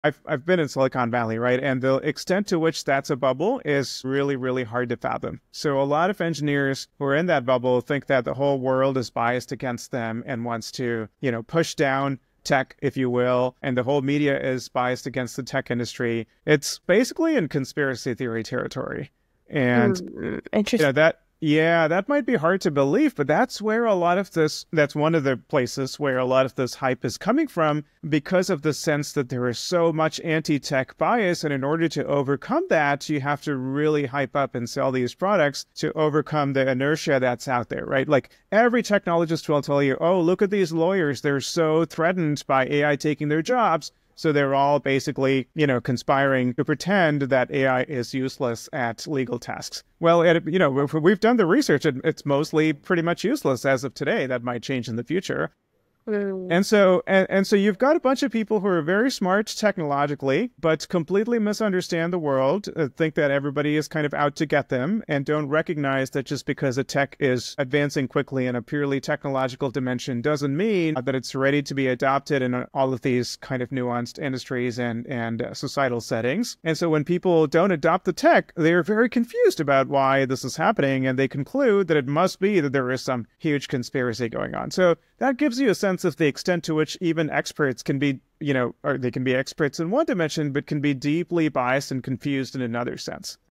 Unpacking the Anti-Tech Conspiracy in Silicon Valley
AI expert Arvind Narayanan explains how hype culture, Silicon Valley bias, and a lack of societal context lead some technologists to believe there's a conspiracy against them. The reality? It’s more complicated than that.